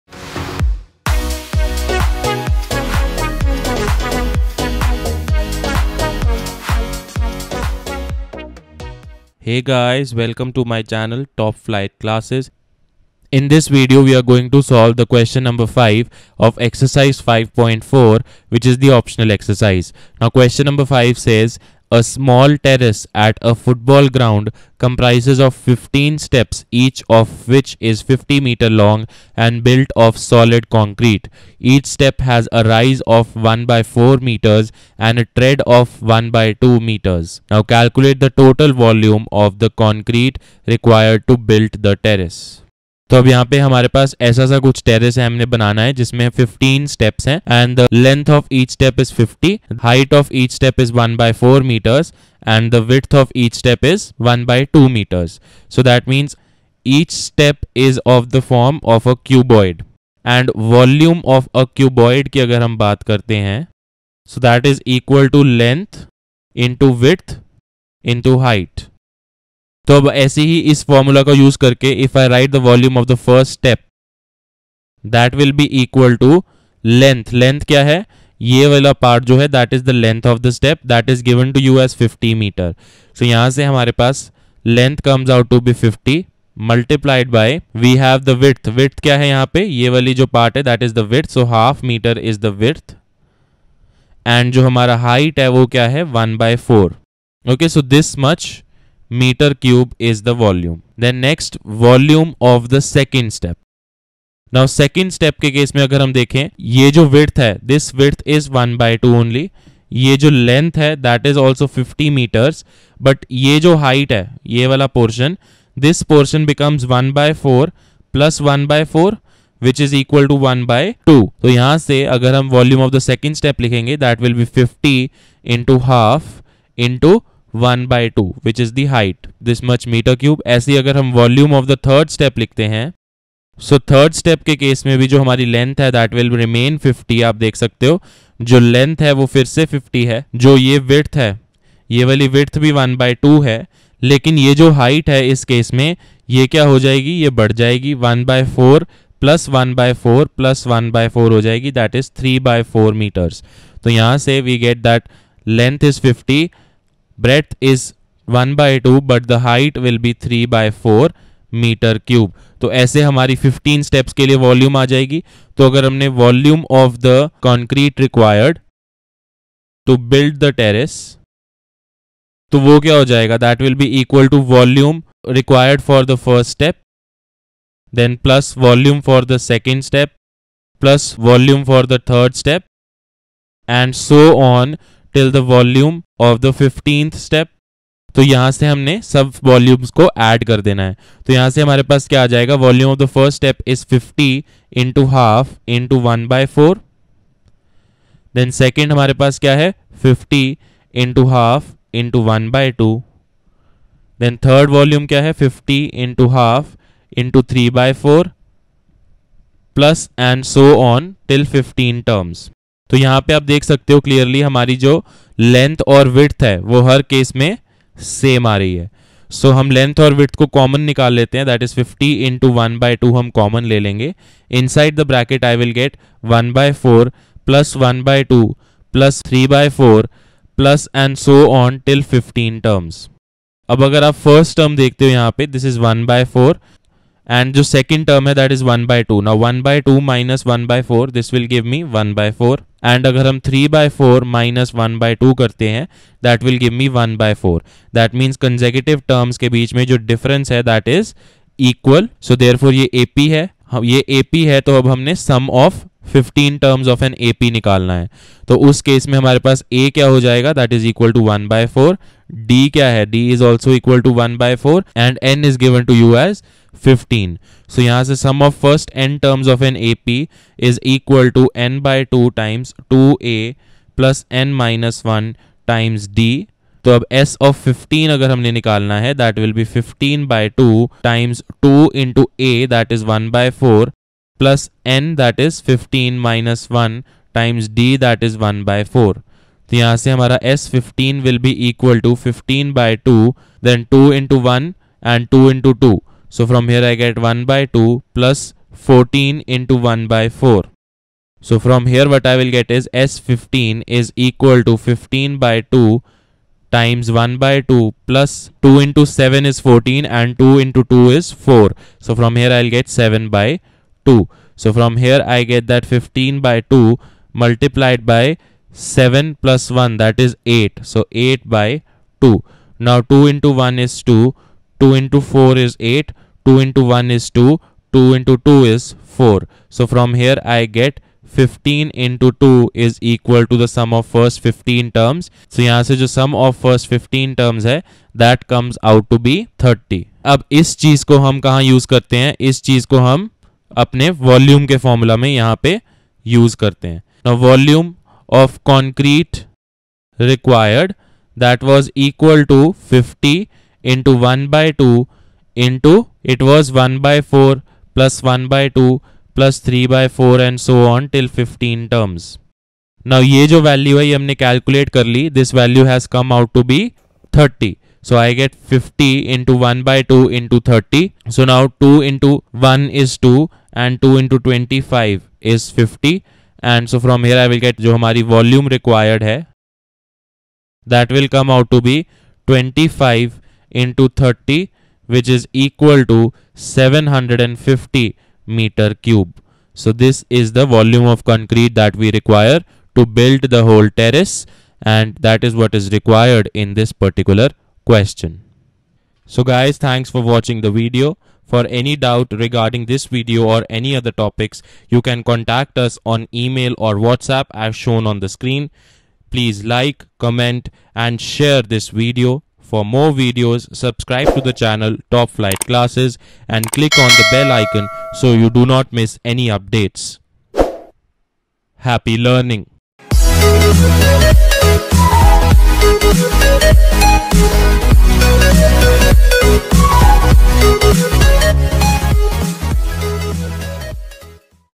Arithmetic Progressions OPTIONAL EXERCISE | Exercise 5.4 Question-5 | Maths Class-10 | Vol.58/58
This video contains the solution of Question-5 of exercise 5.4 (optional exercise) of Arithmetic Progressions Maths Class 10 i.e

A small terrace at a football ground comprises of 15 steps each of which is 50 m long and built of solid concrete.
Each step has a rise of 1/4 m and a tread of 1/2 m. Calculate the total volume of concrete required to build the terrace.